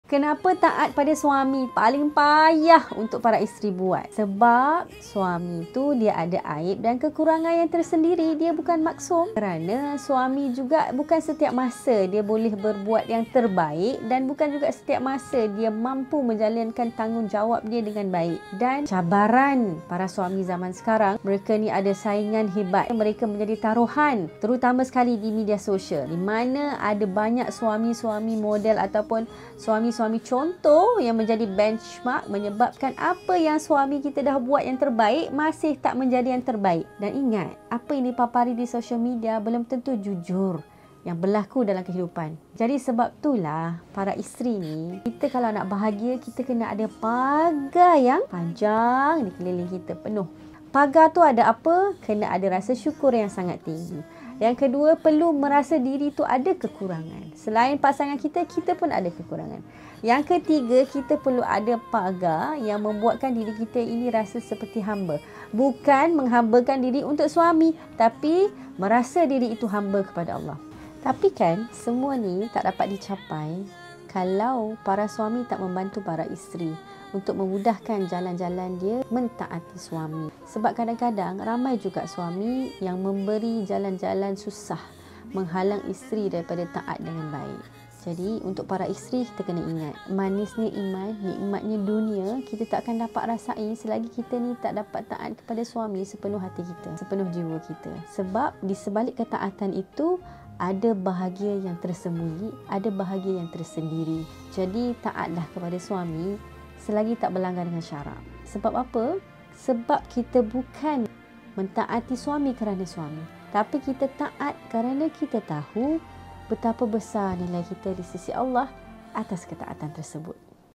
Kenapa taat suami tu payah ? I Bonda Umirah Taib
Taat suami yang paling dituntut tetapi yang paling diuji. Semoga urusan isteri dipermudahkan untuk mentaati dan suami memberi jalan mudah untuk isteri jalani.